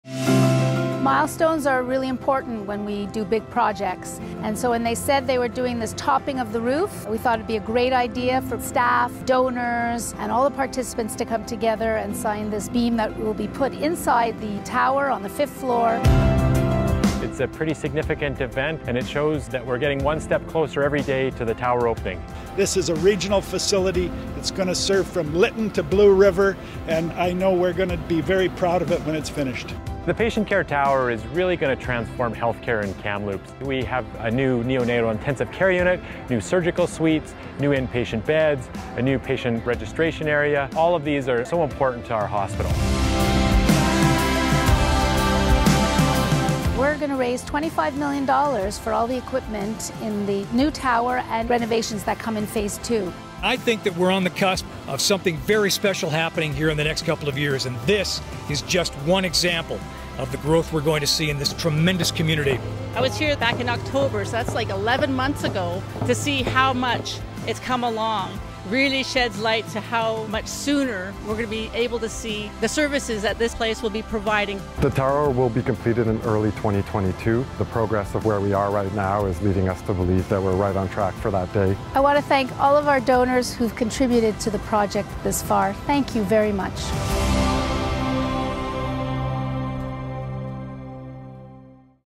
Royal Inland Hospital Patient Care Tower - Beam Signing Event
The RIH Foundation, along with Royal Inland Hospital in Kamloops, EllisDon Infrastructure and Interior Health, hosted a beam signing event for RIH staff, physicians and volunteers, as well as members of the Tk'emlups te Secwépemc, the Two Rivers Metis Association and the Thompson Regional Hospital District. The event took place Sept. 16-18, 2020.